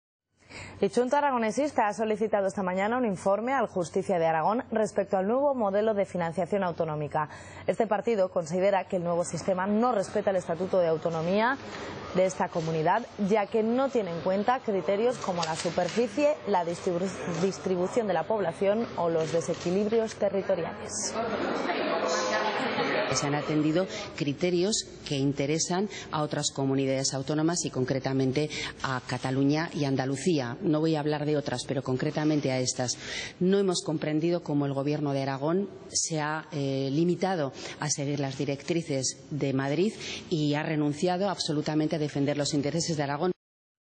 CHA acude al Justicia por la financiación autonómica
Chunta Aragonesista ha solicitado al Justicia de Aragón, Fernando García Vicente, que realice un informe sobre la nueva financiación autonómica. Nieves Ibeas (CHA) ha criticado la postura del Gobierno de Aragón.